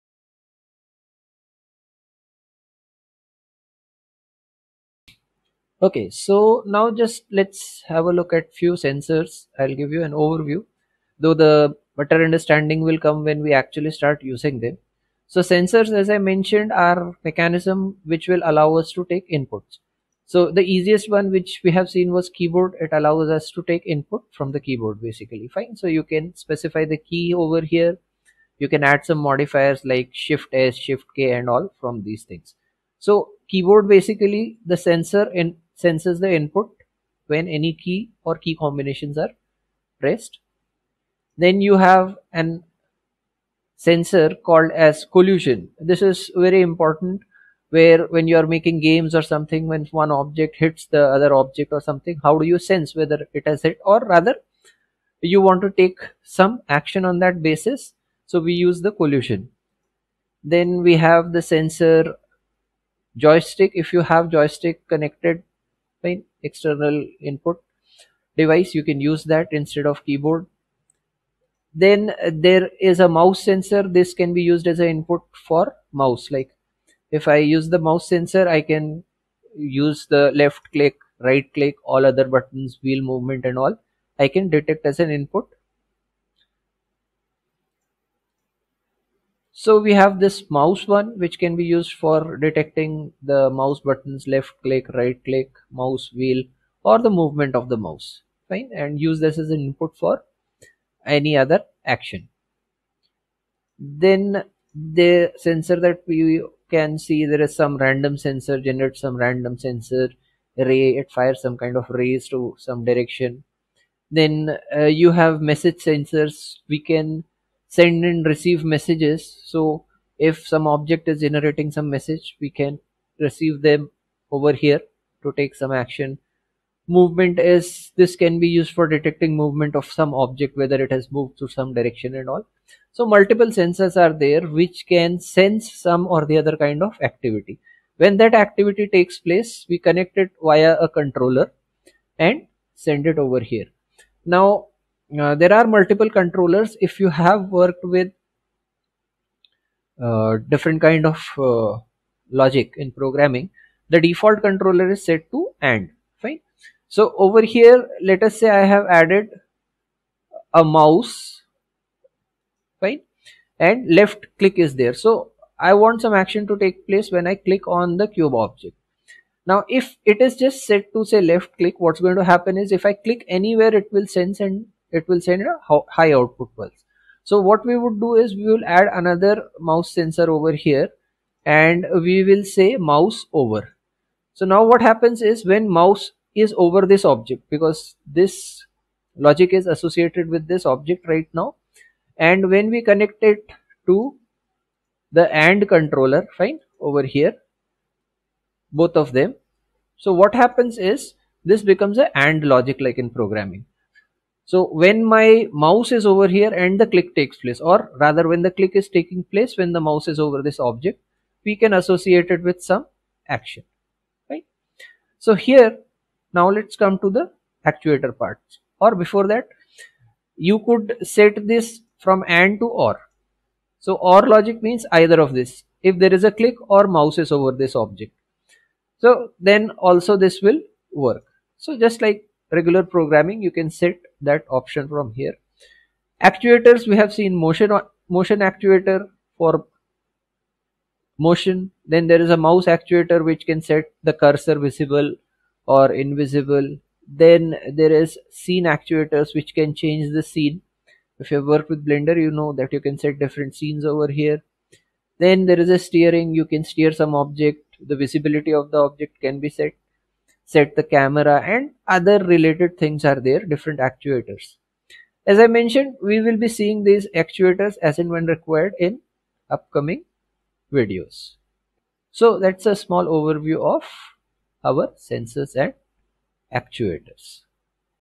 UPBGE Different Sensors and Actuators
While working with UPBGE we will be frequently using the Logic editor. This video gives us the overview different Sensors and actuators